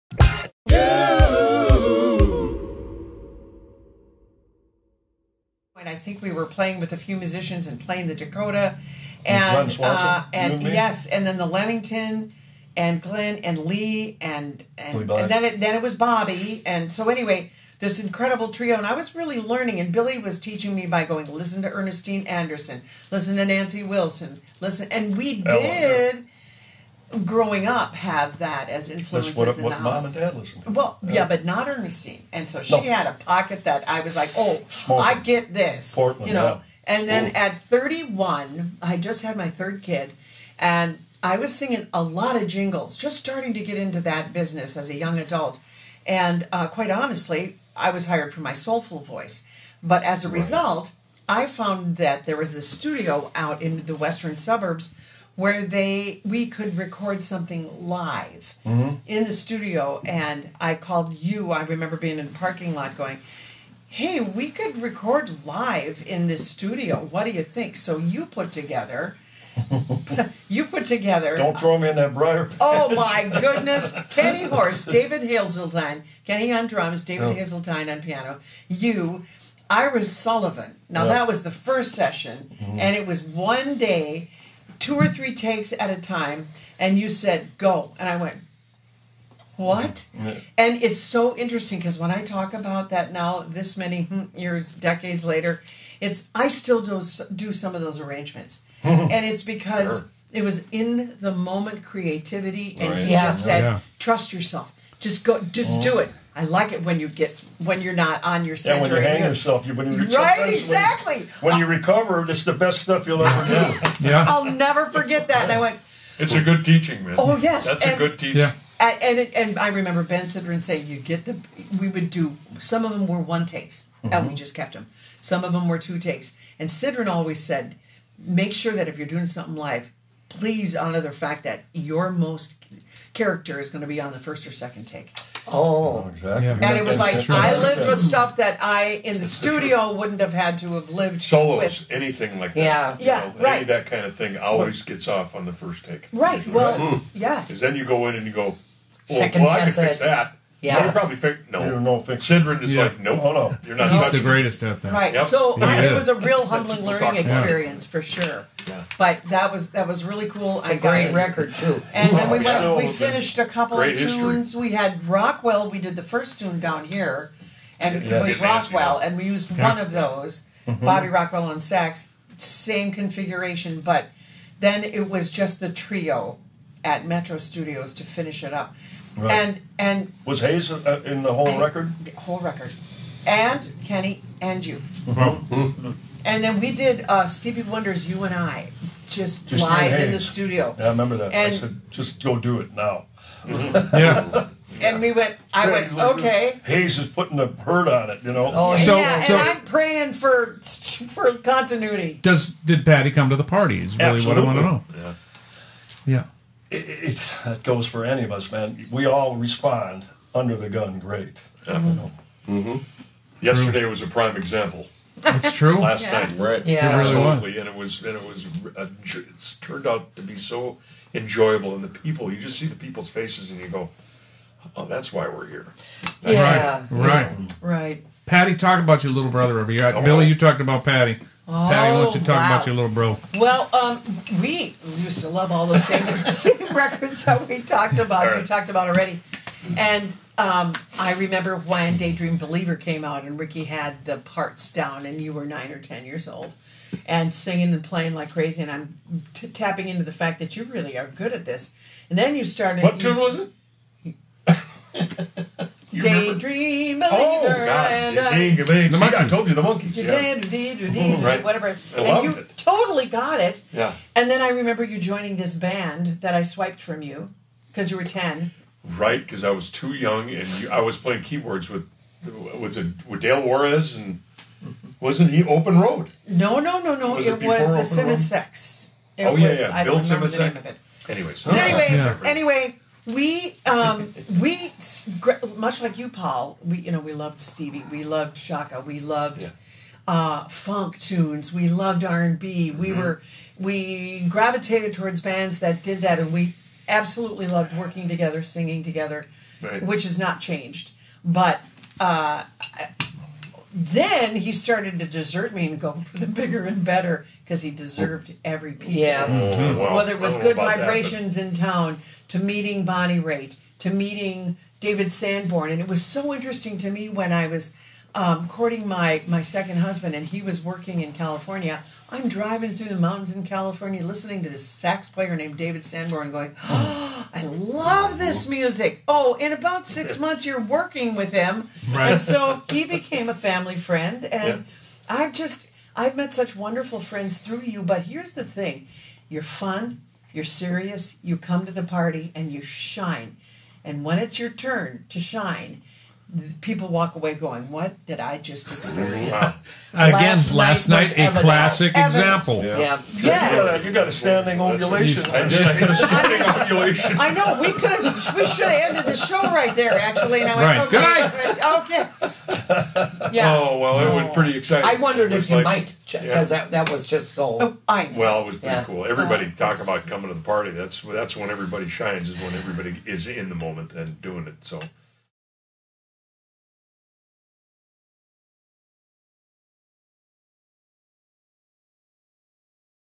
Music in One Take with The Peterson Family
Patty Peterson talks about her experience of making music in the moment and in one take with Ben Sidran as well as her family.